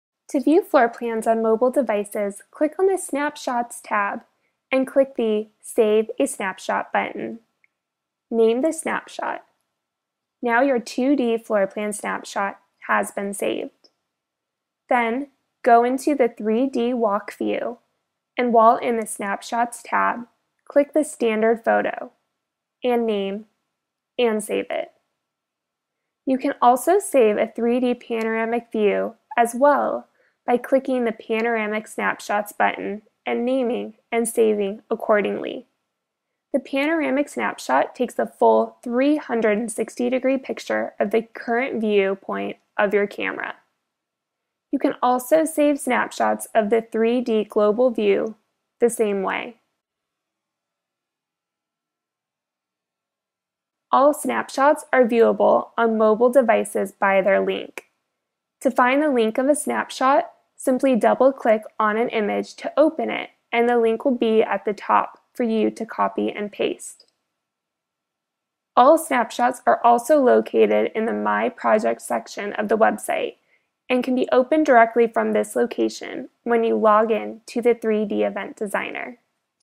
How To: View Floor Plans on Mobile Devices
This is a 3D Event Designer Tutorial that teaches users how to view 2D and 3D interactive floor plans on mobile devices.